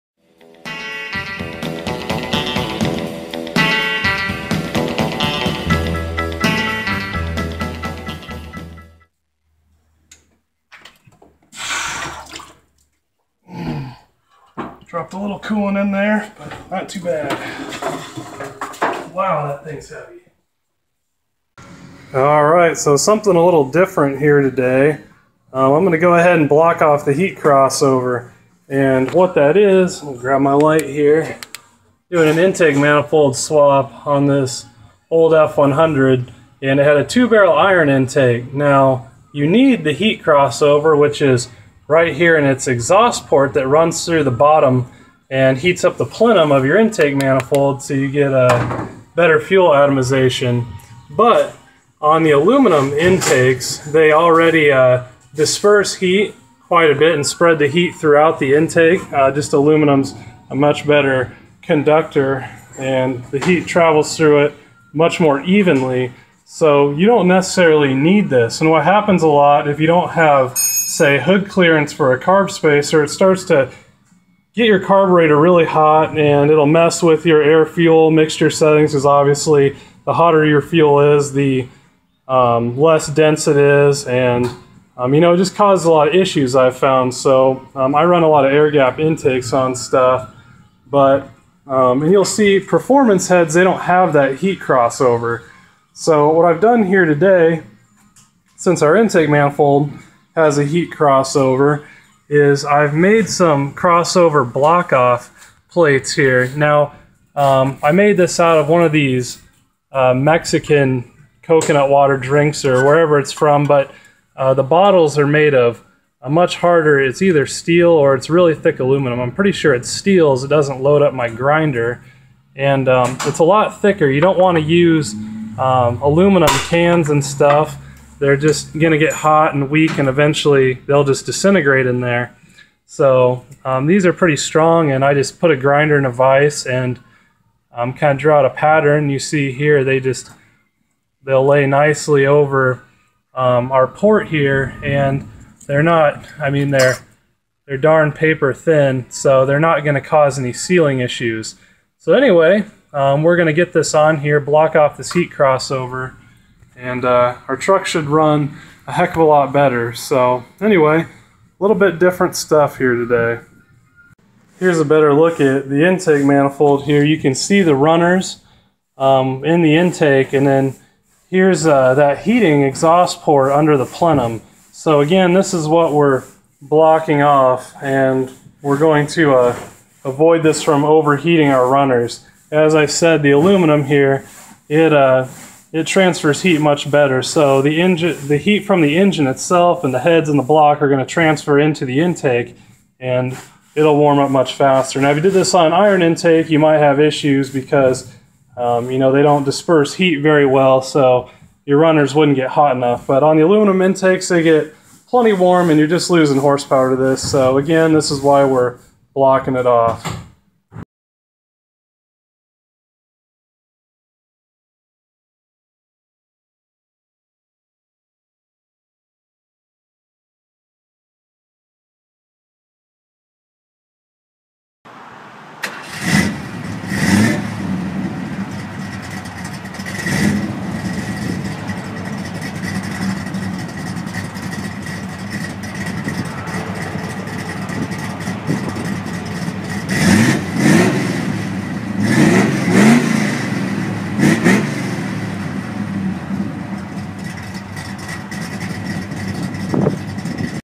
No More Vapor Lock! - Intake Heat Crossover Block Off
cooler and denser air fuel charge for almost nothing! blocking this thing off has always made a huge difference. why do you think performance heads and intakes DO NOT HAVE THEM?!